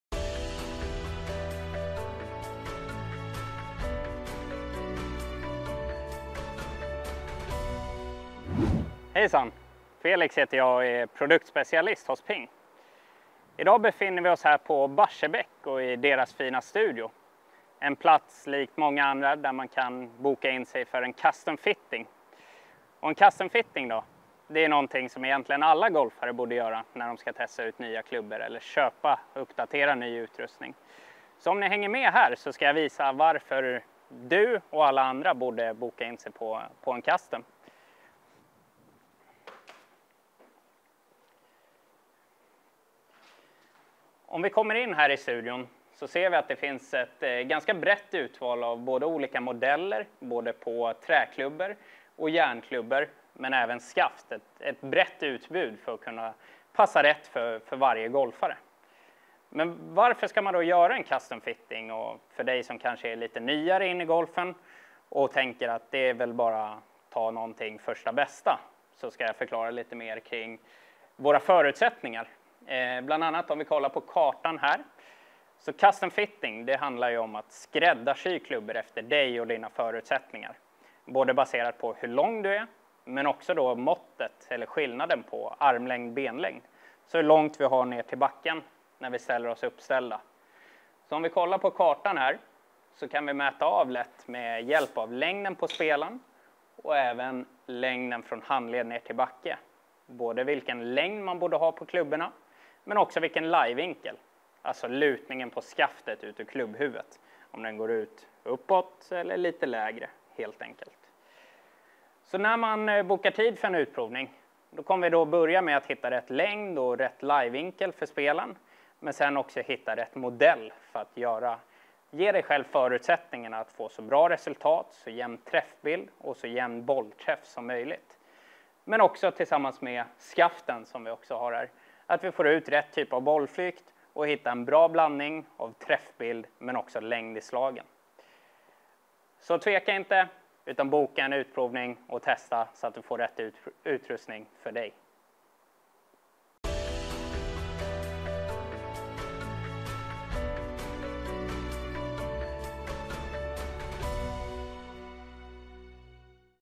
Utrustningstips: Därför ska du göra en utprovning
PINGs produktspecialist Felix Alnefelt berättar hur alla golfare kan dra nytta av en utprovning/custom fitting av utrustning - och ger samtidigt en guidad rundvandring i studion på Barsebäck Resort.